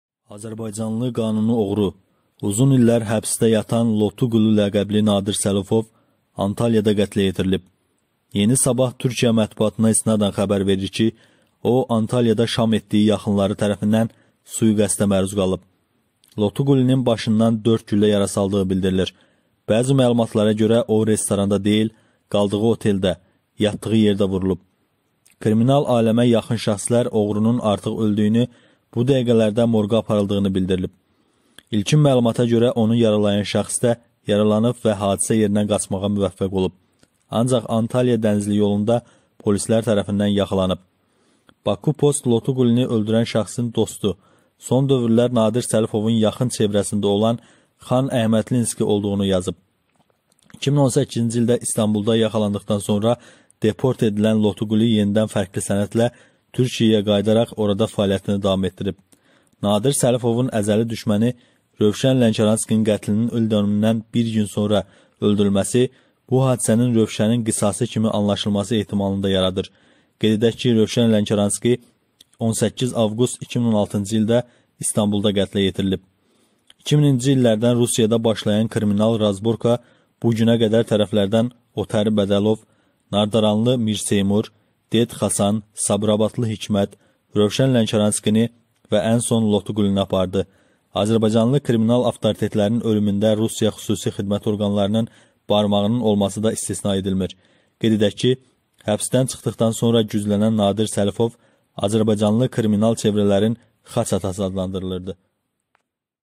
“Lotu Quli” öldürüldü: Başına 4 güllə vurulub
Azərbaycanlı “qanuni oğru”, uzun illər həbsdə yatan “Lotu Quli” ləqəbli Nadir Səlifov Antalyada qətlə yetirilib. “Yeni Sabah” Türkiyə mətbuatına istinadən xəbər verir ki, o, Antalyada şam etdiyi yaxınları tərəfindən sui-qəsdə məruz qalıb.

Loti Qulinin başından 4 güllə yarası aldığı bildirilir. Bəzi məlumatlara görə, o, restoranda deyil, qaldığı oteldə, yatdığı yerdə vurlub.

Kriminal aləmə yaxın şəxslər oğrunun artıq öldüyünü, bu dəqiqələrdə morqa aparıldığını bildirir.

İlkin məlumata görə onu yaralayan şəxs də yaralanıb və hadisə yerindən qaçmağa müvəffəq olub. Ancaq Antalya-Dənizli yolunda polislər tərəfindən yaxalanıb.

BakuPost Lotu Qulini öldürən şəxsin  dostu, son dövrlər Nadir Səlifovun yaxın çevrəsində olan Xan Əhmədlinski olduğunu yazıb.

2018-ci ildə İstanbulda yaxalandıqdan sonra deport edilən Lotu Quli yenidən fərqli sənədlə Türkiyəyə qayıdaraq, orada fəaliyyətini davam etdirib.

Nadir Səlifovun əzəli düşməni Rövşən Lənkəranskinin qətlinin ildönümündən bir gün sonra (R.Lənkəranski 18 avqust 2016-cı ildə İstanbulda qətlə yetirilib) öldürülməsi bu hadisənin Rövşənin qisası kimi anlaşılması ehtimalını da yaradır. 2000-ci illərdən Rusiyada başlayan kriminal “razborka” bu günə qədər tərəflərdən Otari Bədəlov, nardaranlı Mirseymur, Ded Xasan, sabirabadlı Hikmət, Rövşən Lənkəranskini və ən son Lotu Qulini “apardı”.

Azərbaycanlı kriminal avtoritetlərin ölümündə Rusiya xüsusi xidmət orqanlarının barmağının olması da istisna edilmir.

Qeyd edək ki, həbsdən çıxdıqdan sonra güclənən Nadir Səlifov azərbaycanlı kriminal çevrələrin “Xaç atası” adlandırılırdı.